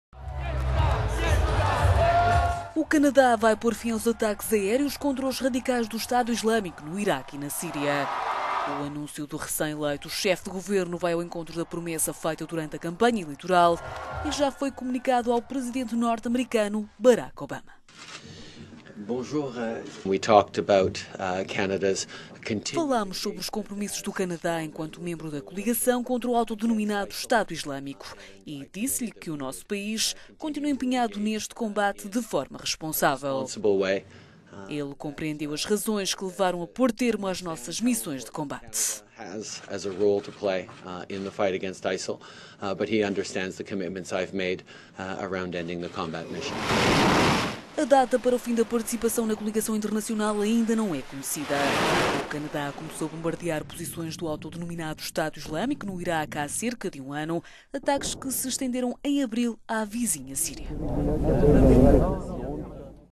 Canadá vai acabar com ataques aéreos contra radicais do ISIL
O Canadá vai por fim aos ataques aéreos contra os radicais do Estado Islâmico no Iraque e na Síria.

O anúncio do recém-eleito chefe de governo vai ao encontro da promessa feita durante a campanha eleitoral e já foi comunicado ao presidente norte-americano, Barack Obama.

"Falamos sobre os compromissos do Canadá enquanto membro da coligação contra o autodenominado Estado Islâmico e disse-lhe que o nosso país continua empenhado neste combate de forma responsável. Ele compreendeu as razões que l…